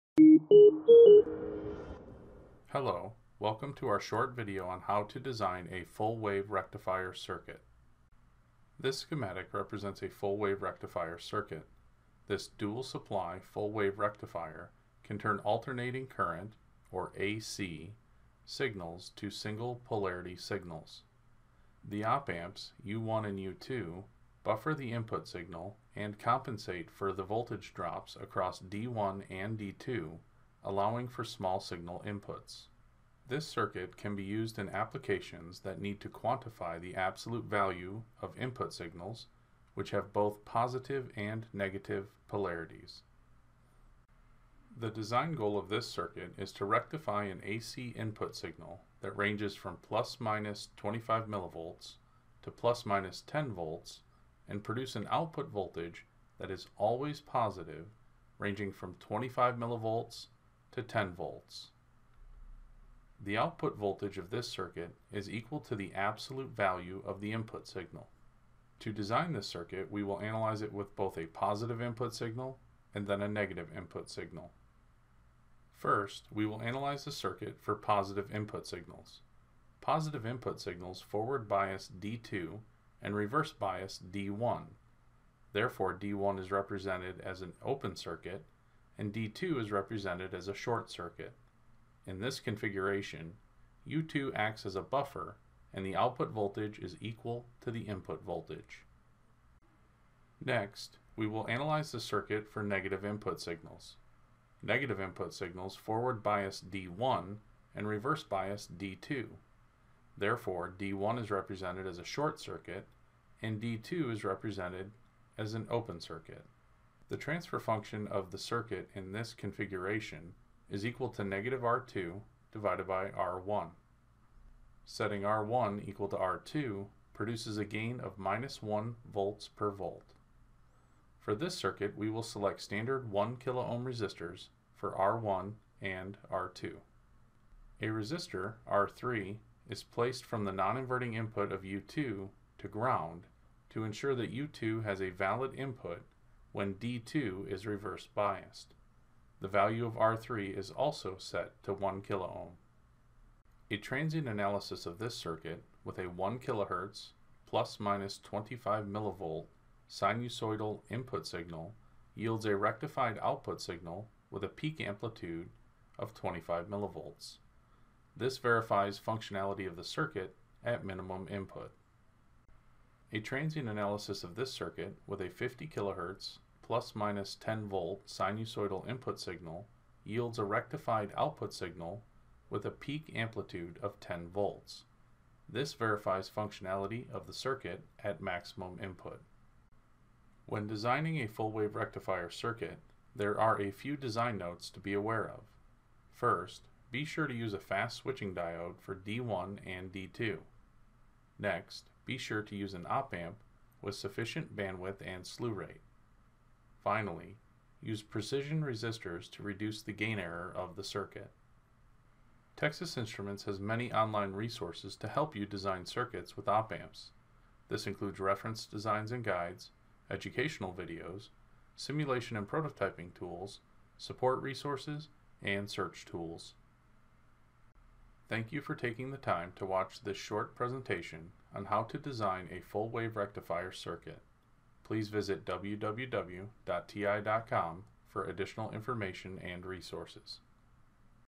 Full-wave rectifier circuit
This absolute value circuit can turn alternating current (AC) signals to single polarity signals. This circuit functions with limited distortion for ±10-V input signals at frequencies up to 50kHz and for signals as small as ±25mV at frequencies up to 1kHz.